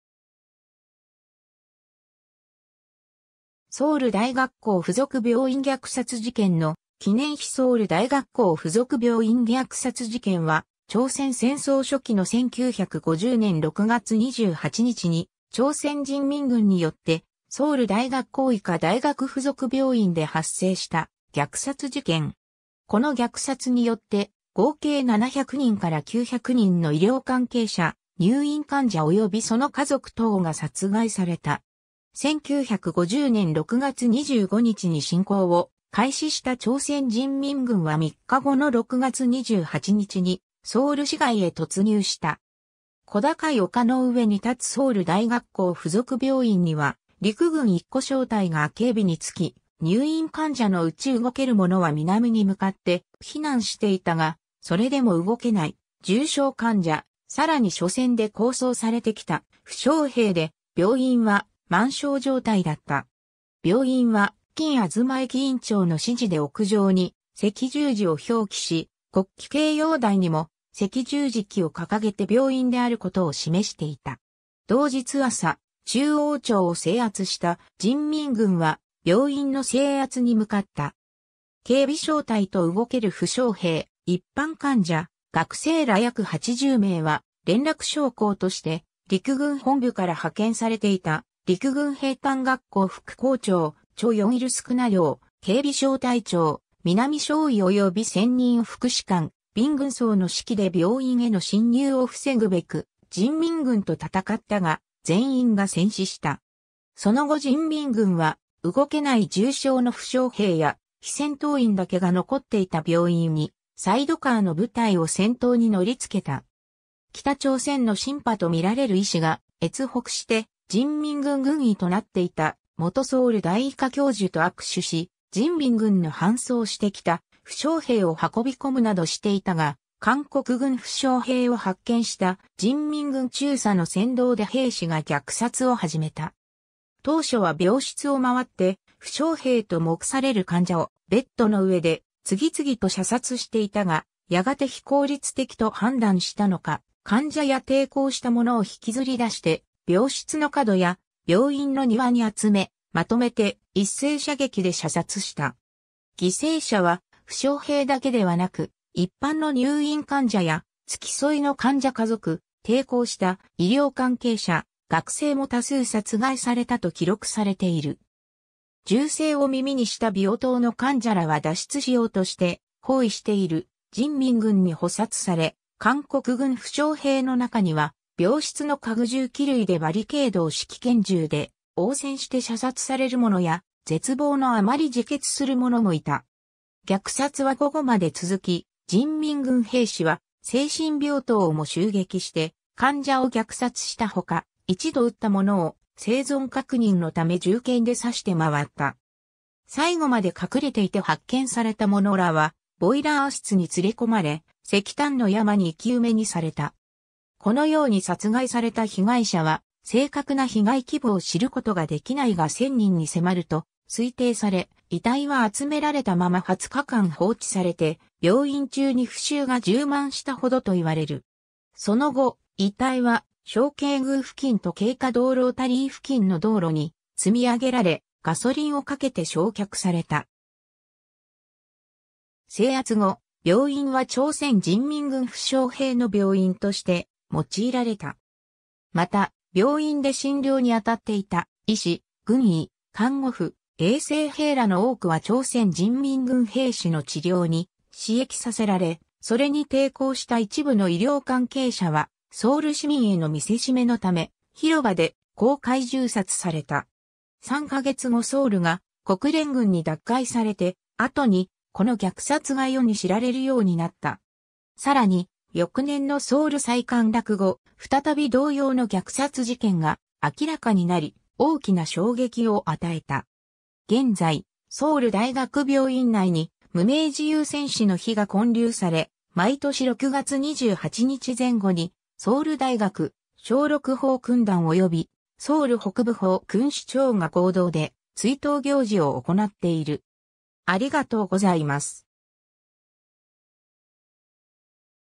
ソウル大学校付属病院虐殺事件
ソウル大学校附属病院虐殺事件の記念碑 ソウル大学校附属病院虐殺事件 (ソウルだいがっこうふぞくびょういんぎゃくさつじけん、朝鮮語: 서울대학교 부속병원 학살 사건 韓文漢字: 서울大學校附属病院虐殺事件 中国語：漢城國立大學附屬醫院屠殺事件)は朝鮮戦争初期の1950年6月28日に，朝鮮人民軍によってソウル大学校医科大学附属病院（現・ソウル大学校病院）で発生した虐殺事件。
この虐殺によって合計700人から900人の医療関係者，入院患者及びその家族等が殺害された。
 1950年6月25日に侵攻を開始した朝鮮人民軍は3日後の6月28日にソウル市街へ突入した（第一次ソウル会戦）。
小高い丘の上に建つソウル大学校附属病院には陸軍1個小隊が警備に就き，入院患者のうち動ける者は南に向かって避難していたが，それでも動けない重症患者，さらに緒戦で後送されてきた負傷兵で病院（病棟4棟，約800床）は満床状態だった。
病院は金東益院長の指示で屋上に赤十字を表記し，国旗掲揚台にも赤十字旗を掲げて病院であることを示していた。
 同日朝，中央庁（旧朝鮮総督府庁舎）を制圧した人民軍は病院の制圧に向かった。
警備小隊と動ける負傷兵・一般患者・学生ら約80名は，連絡将校として陸軍本部から派遣されていた陸軍兵站学校副校長チョ・ヨンイル少領（少佐），警備小隊長・南少尉及び先任副士官（小隊軍曹）・閔軍曹の指揮で病院への侵入を防ぐべく人民軍と戦ったが，全員が戦死した。
 その後人民軍は，動けない重傷の負傷兵や非戦闘員だけが残っていた病院にサイドカーの部隊を先頭に乗り付けた。
北朝鮮のシンパとみられる医師が，越北して人民軍軍医となっていた元ソウル大医科教授と握手し，人民軍の搬送してきた負傷兵を運び込むなどしていたが，韓国軍負傷兵を発見した人民軍中佐の扇動で兵士が虐殺を始めた。
当初は病室をまわって負傷兵と目される患者をベッドの上で次々と射殺していたが，やがて非効率的と判断したのか患者や抵抗した者を引き摺り出して病室の角や病院の庭に集め，纏めて一斉射撃で射殺した。
犠牲者は負傷兵だけではなく，一般の入院患者や，付き添いの患者家族，抵抗した医療関係者・学生も多数殺害されたと記録されている。
 銃声を耳にした病棟の患者らは脱出しようとして包囲している人民軍に捕殺され，韓国軍負傷兵の中には病室の家具什器類でバリケードを敷き拳銃で応戦して射殺される者や，絶望のあまり自決する者もいた。
 虐殺は午後まで続き，人民軍兵士は精神病棟をも襲撃して患者を虐殺したほか，一度撃った者を生存確認のため銃剣で刺してまわった。
 最後まで隠れていて発見された者らはボイラー室に連れ込まれ，石炭の山に生き埋めにされた。
 このように殺害された被害者は、正確な被害規模を知ることができないが1000人に迫ると推定され（犠牲者は一般市民だけで900人に達し，韓国国防部によれば、犠牲者のうち100人が負傷兵だという）、遺体は集められたまま20日間放置されて、病院中に腐臭が充満したほどと言われる。
その後，遺体は昌慶宮付近と恵化洞ロータリー付近の道路に積み上げられ，ガソリンを掛けて焼却された。
 制圧後，病院は朝鮮人民軍負傷兵の病院として用いられた。
また，病院で診療に当たっていた医師・軍医，看護婦，衛生兵らの多くは朝鮮人民軍兵士の治療に使役させられ，それに抵抗した一部の医療関係者はソウル市民への見せしめのため，広場で公開銃殺された。
 3か月後ソウルが国連軍に奪回されて（第二次ソウル会戦）後に，この虐殺が世に知られるようになった。
さらに，翌年のソウル再陥落（第三次ソウル会戦）後，再び同様の虐殺事件が明らかになり，大きな衝撃を与えた。
 現在，ソウル大学病院内に「無名自由戦士の碑」が建立され，毎年6月28日前後に，ソウル大学，鍾路区報勲団及びソウル北部報勲支庁が合同で追悼行事を行っている。